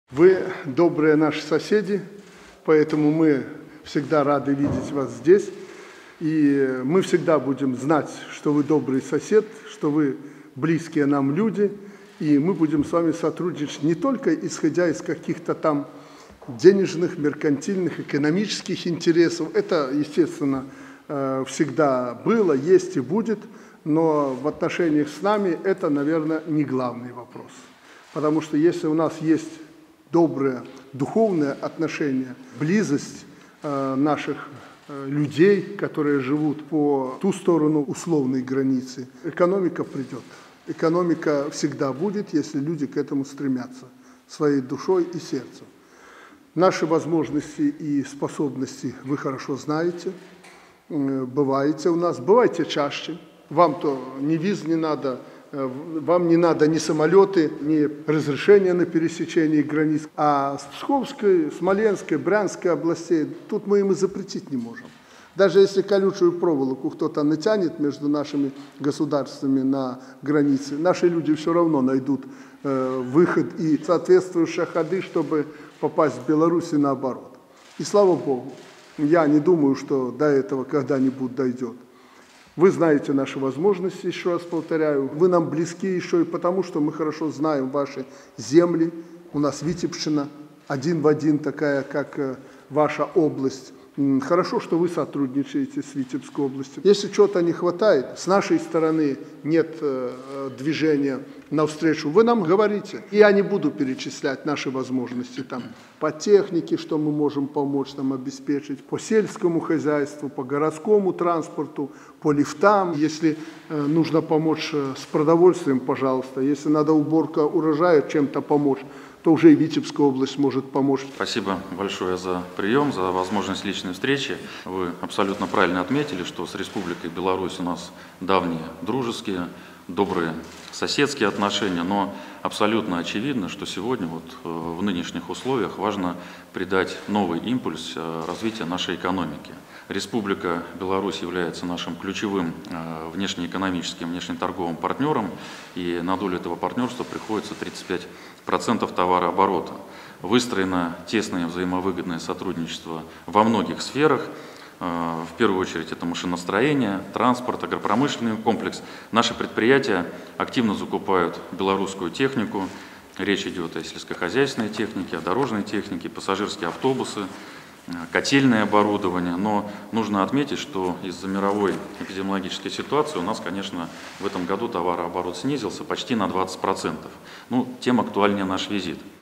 Лукашенко - губернатору Псковской области: если у наших людей добрые отношения, экономика придет
Беларусь готова развивать сотрудничество с Псковской областью не только из-за денежных интересов, но и духовной близости между людьми. Об этом Президент Беларуси Александр Лукашенко заявил на встрече с губернатором Псковской области России Михаилом Ведерниковым.